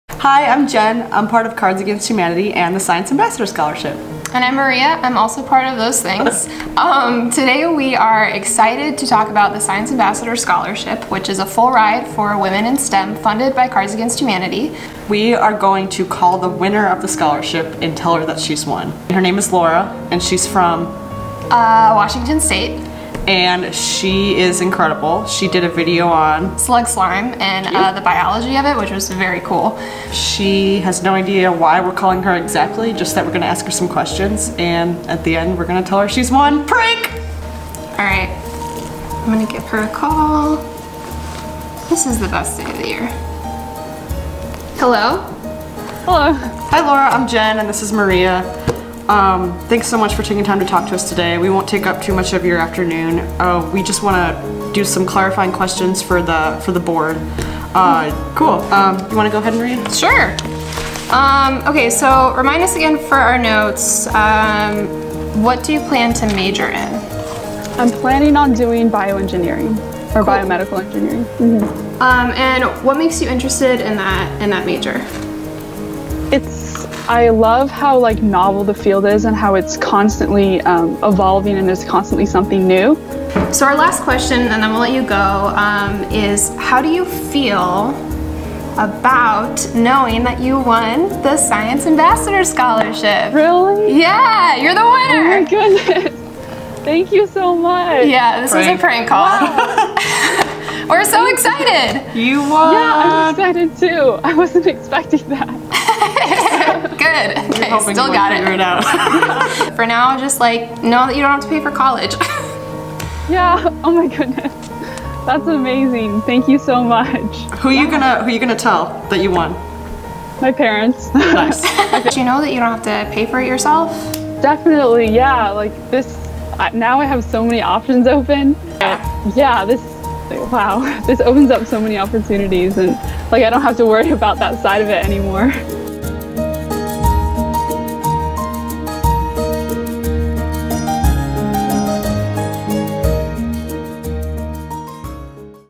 Cards Against Humanity awards full-ride scholarship
The Science Ambassador Scholarship is a full-ride scholarship for women in science, technology, engineering, or math, funded by Cards Against Humanity. Laura Chen, a high school senior headed to the Massachusetts Institute of Technology this fall, won the 2018 award.